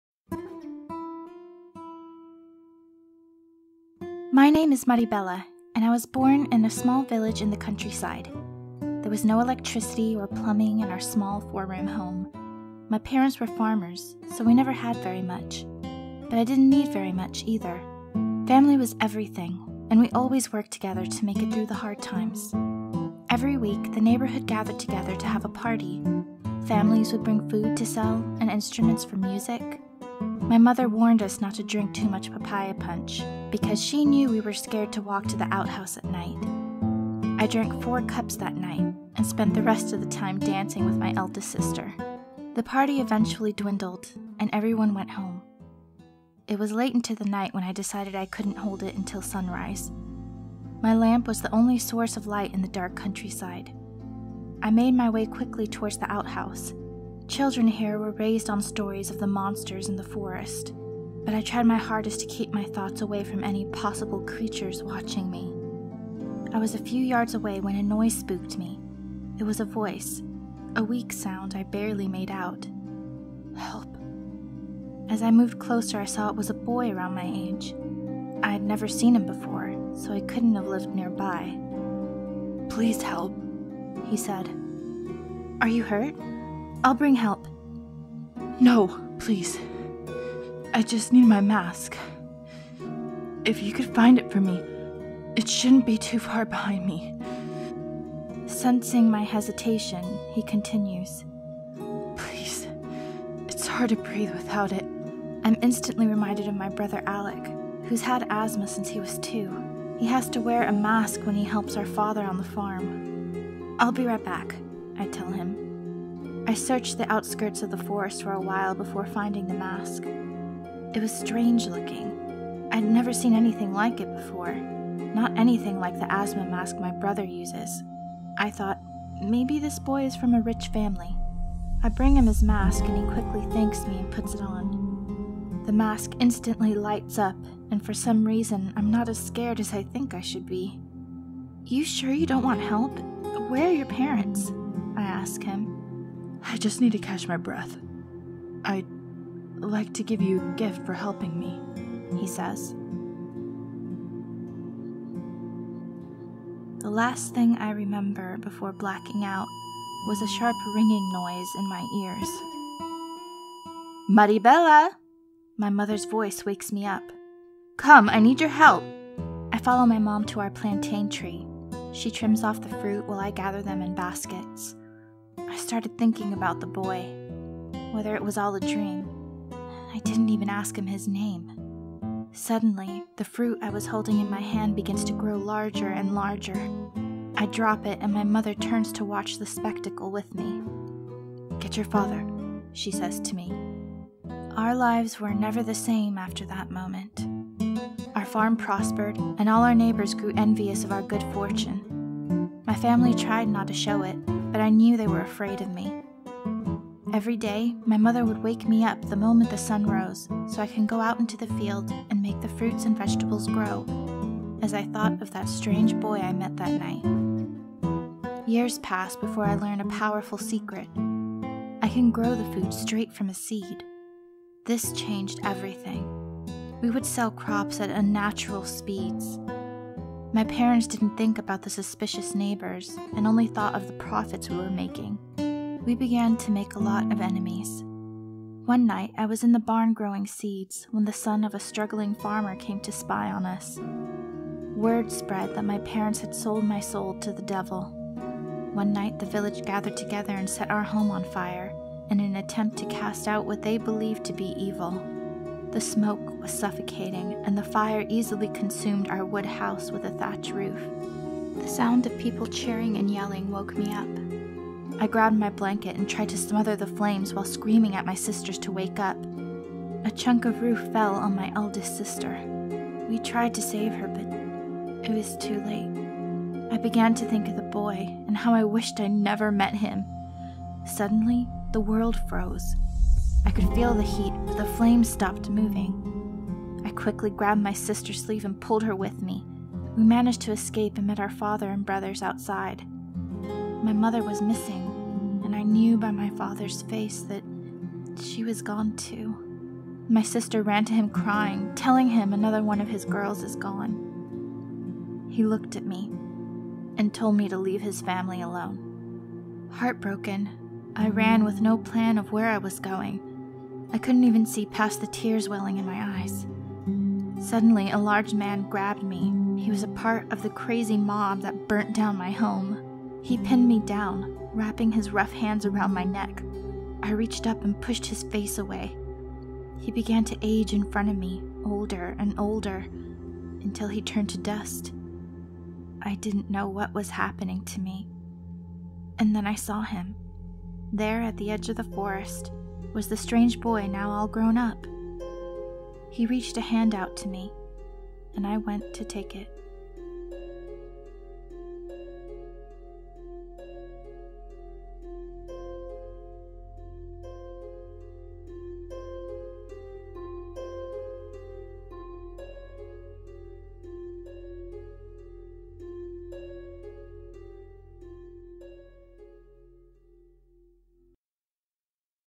Creatures in the Forest by Fenweh Studios
Judges pick for top video in the 'Create a Superhero' contest administered by the Passion Competition.

Creatures in the Forest
Maribela meets a creature of the forest and receives the gift of time.

Animated by Fenweh Studios using Adobe Flash.
Narrated by Sonya.